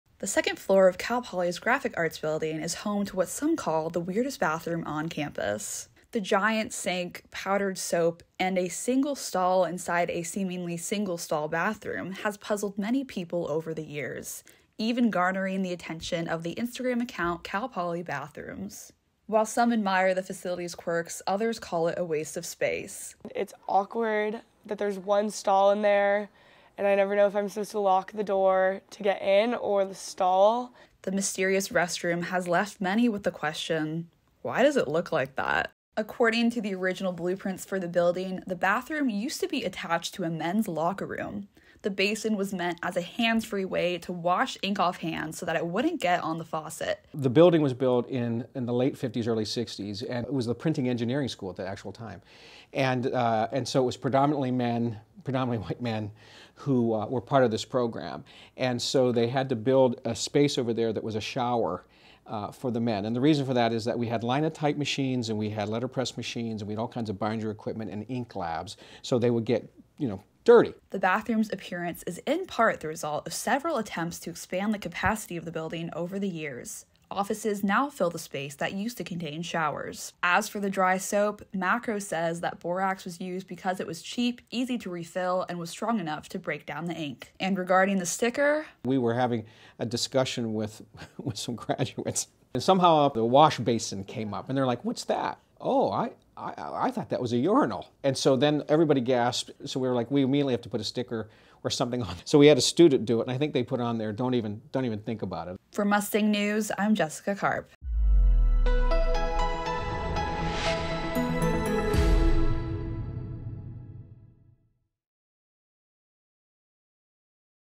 The Weirdest Bathroom on Cal Poly's Campus
Discover the mystery behind Cal Poly's quirkiest bathroom in our latest investigative report! Reporter Jessica Carp delves into the history and peculiarities of this campus curiosity.